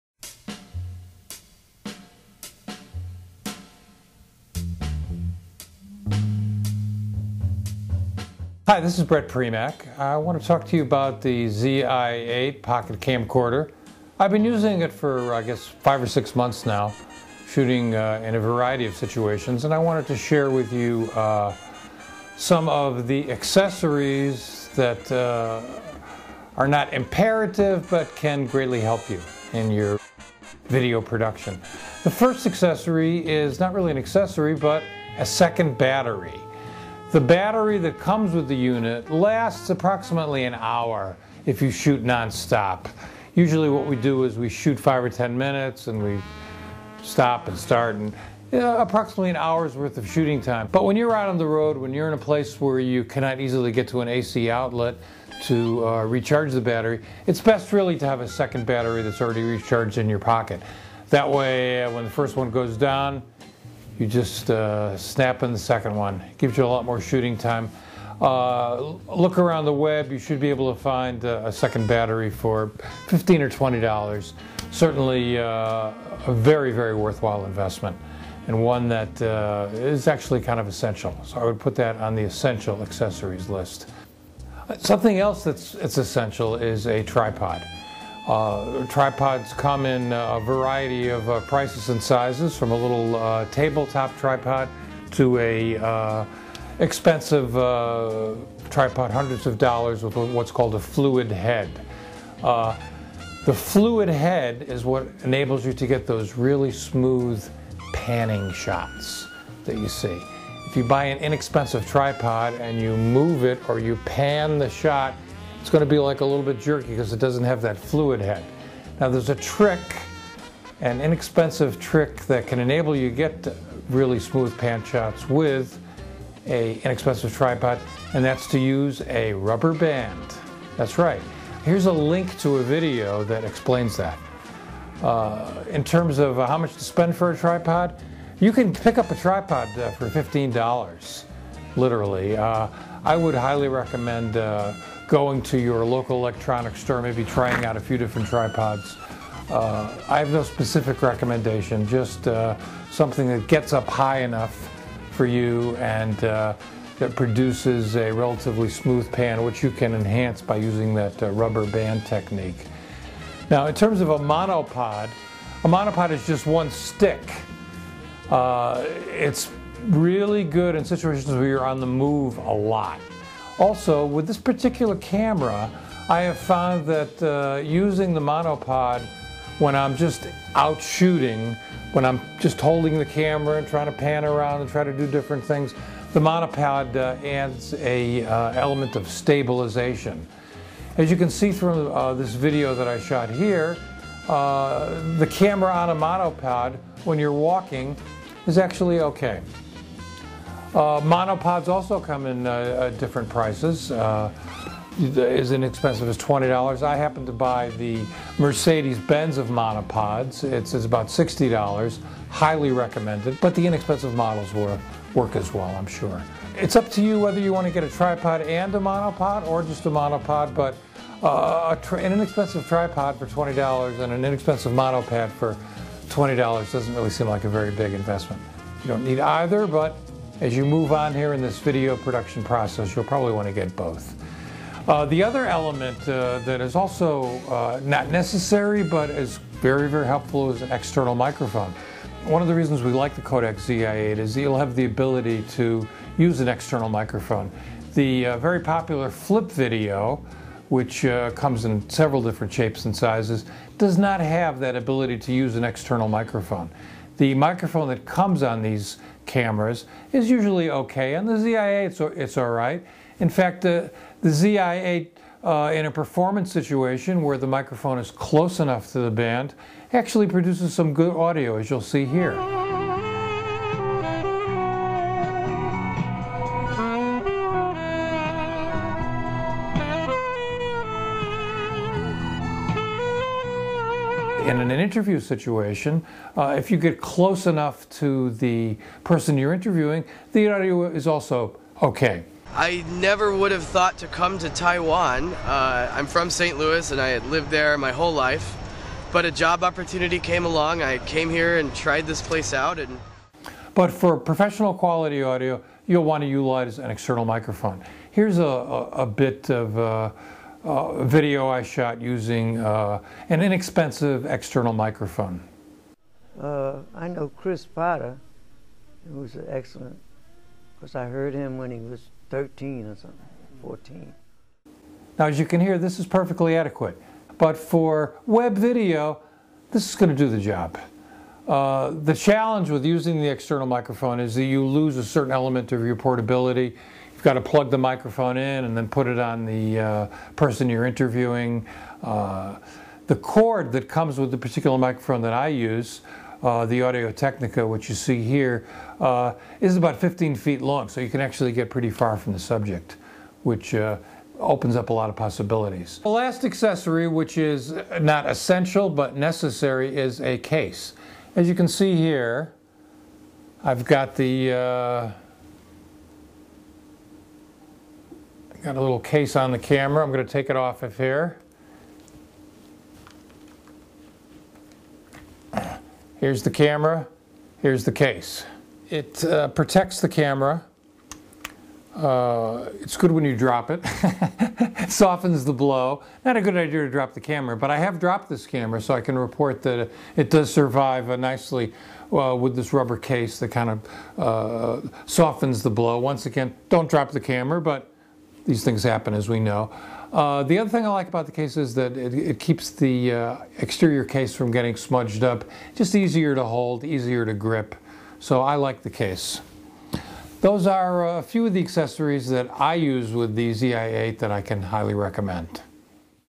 Zi8 Accessories
Bret talks about some essential and nearly essential accessories for the Kodak Zi8.